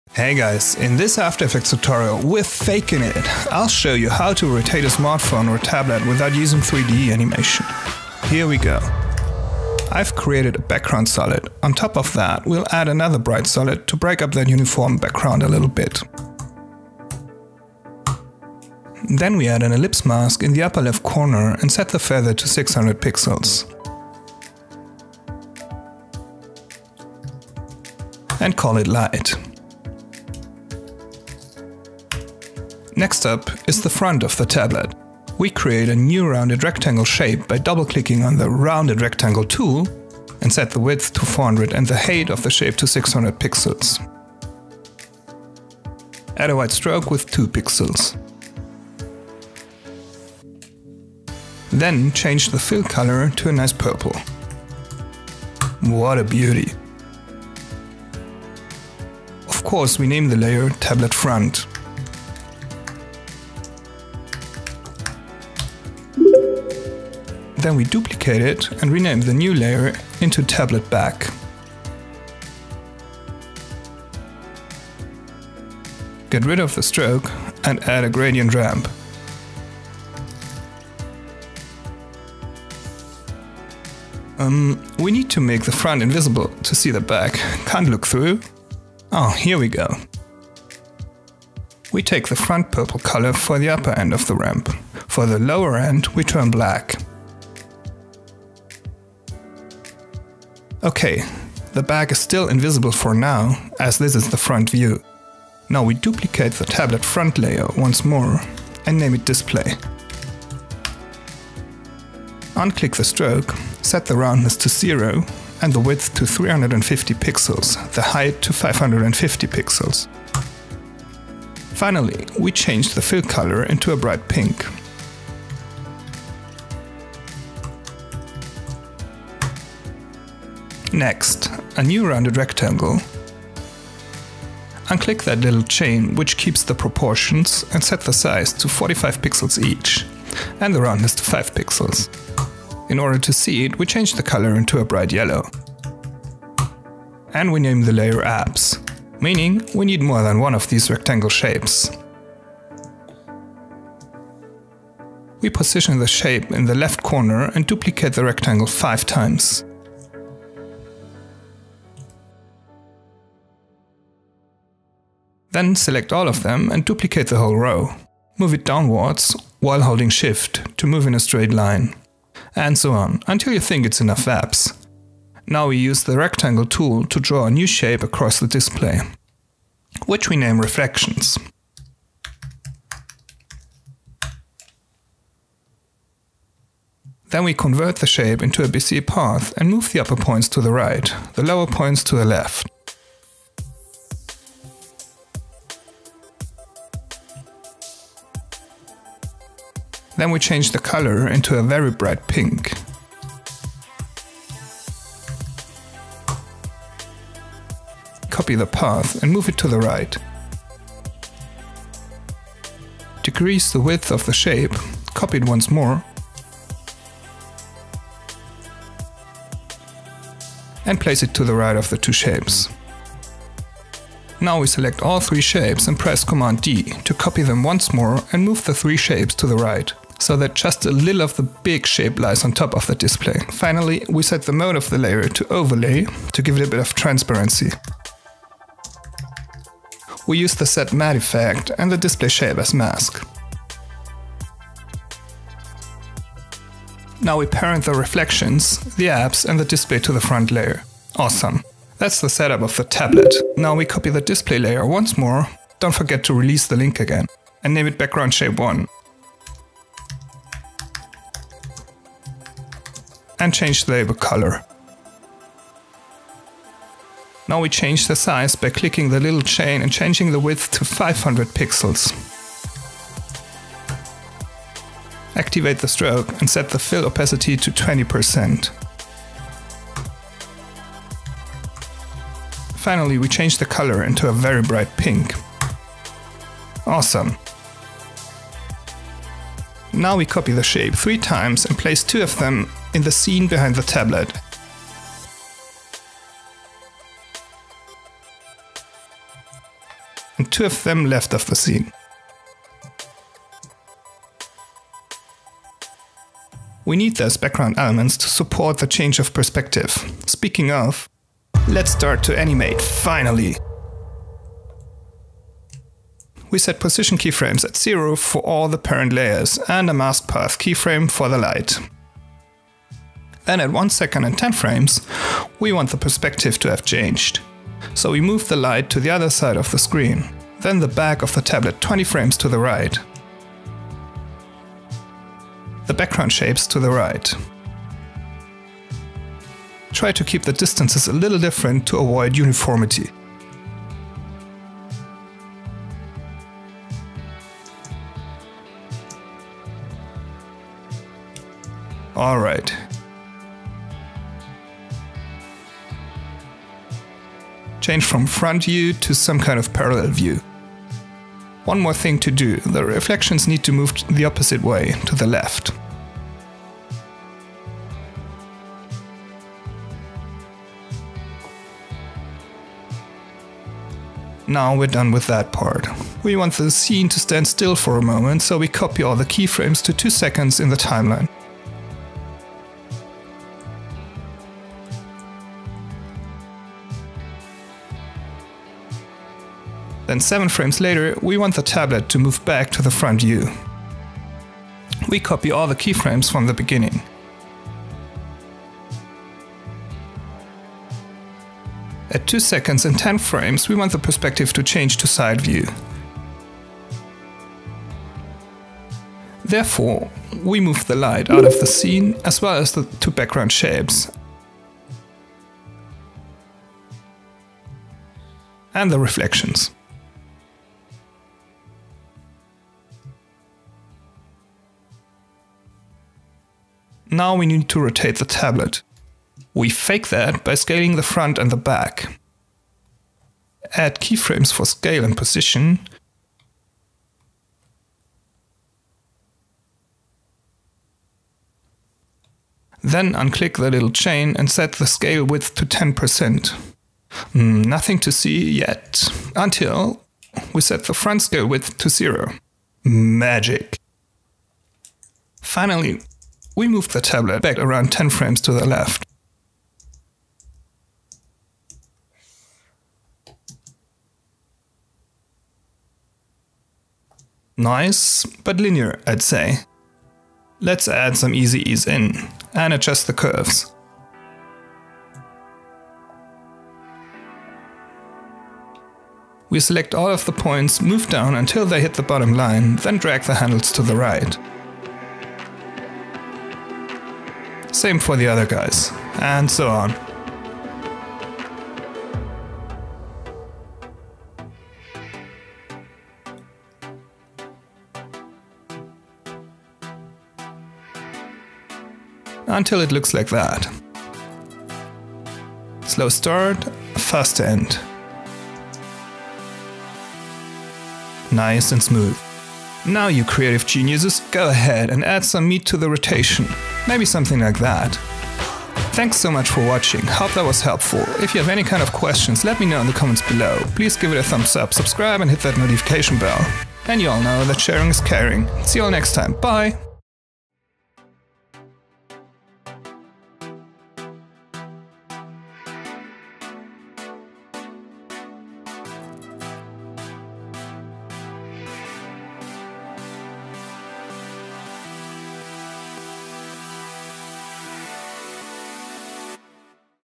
Fake 3D rotation in After Effects Tutorial
In this tutorial, I'll show you how to easily use fake 3D animation in After Effects. We rotate a tablet without using 3D animation.

We change the  perspective by moving shape layers around and by simply scaling the width of shape layers.

The faux 3D rotation is supported by reflections which are moving over the display.
Additionally we add some background shapes and a light, which rotate, together with the tablet.

After creating the tablet with shape layers, we animate the rotation from front view to parallel view and then finally to side view.
Then we add easy ease in and use the graph editor to smooth out the movements.